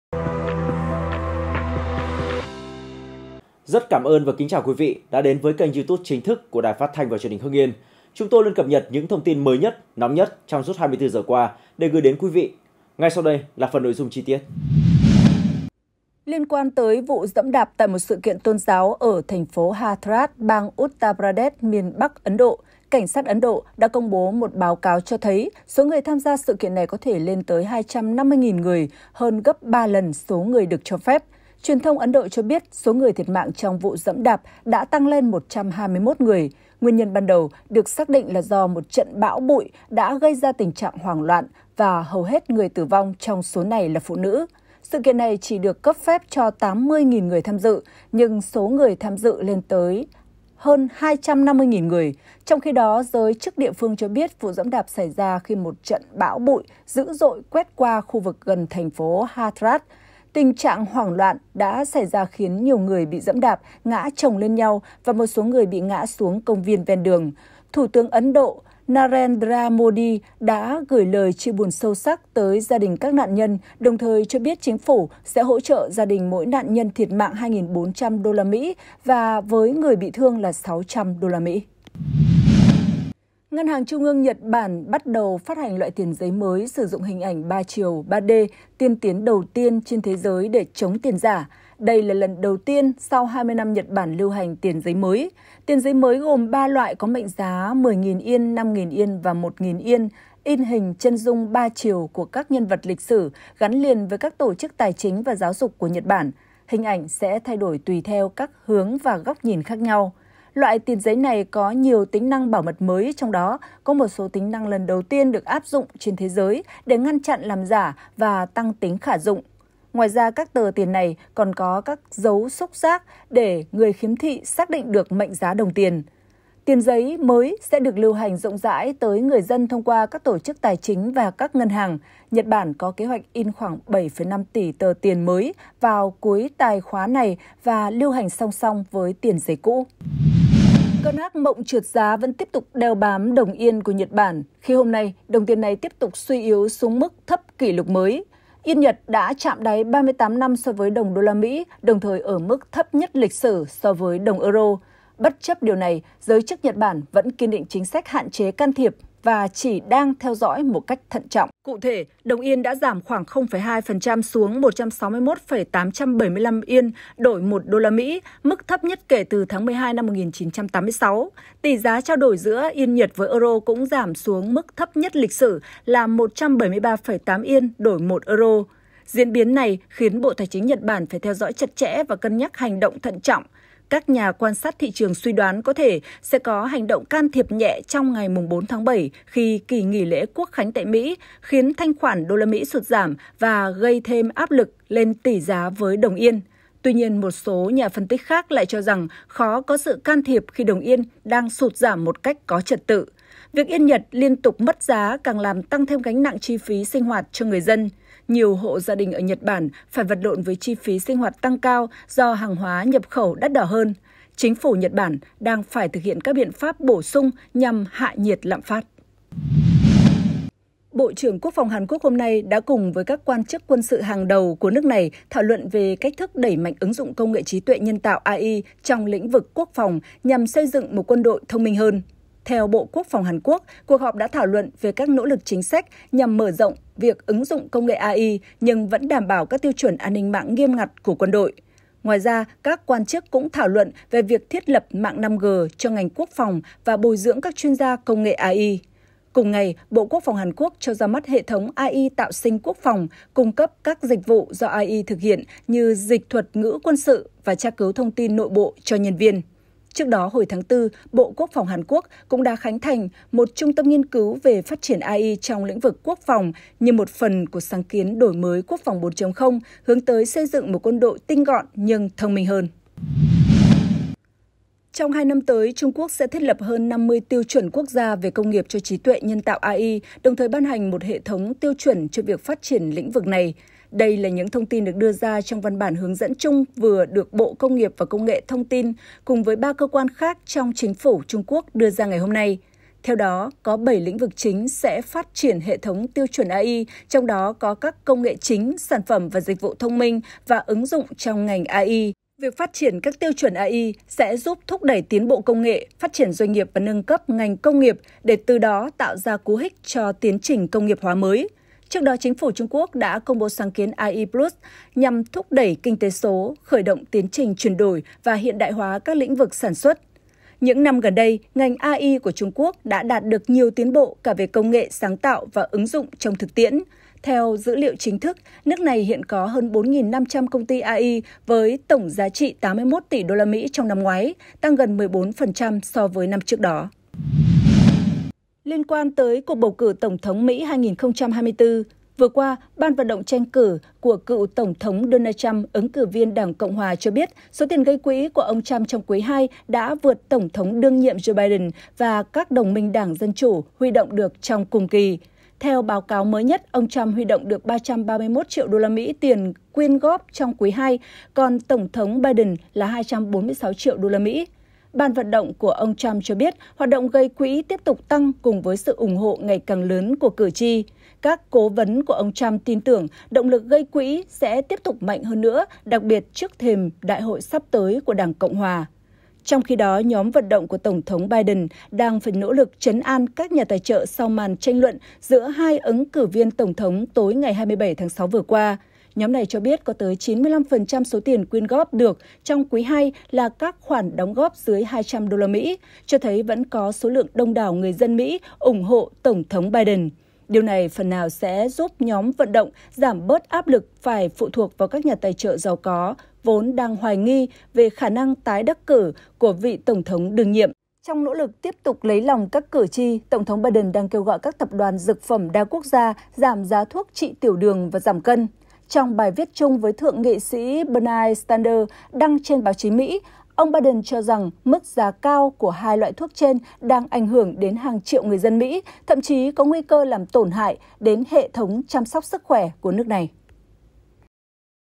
Trung Quốc thiết lập trên 50 tiêu chuẩn về AI -Tiền giấy sử dụng công nghệ 3D đầu tiên trên thế giới
Hưng Yên TV | Trung Quốc thiết lập trên 50 tiêu chuẩn về AI -Tiền giấy sử dụng công nghệ 3D đầu tiên trên thế giới
Địa chỉ: 164 Nguyễn Văn Linh, thành phố Hưng Yên, tỉnh Hưng Yên
Tiktok: Truyền hình Hưng Yên
Facebook: Đài Phát thanh và Truyền hình Hưng Yên
Zalo: Đài PTTH Hưng Yên